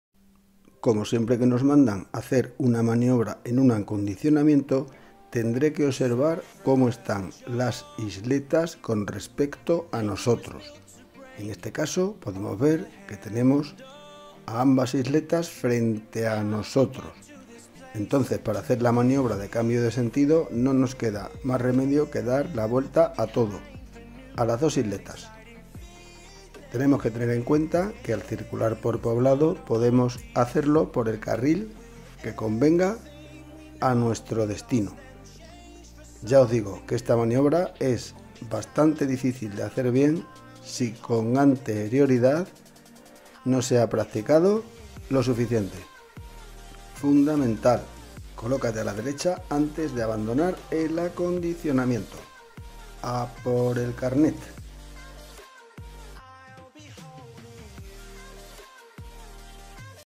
Acondicionamientos (5/6)
Aprende a hacer un cambio de sentido en un acondicionamiento. Es una maniobra que hay que practicar para realizarla correctamente.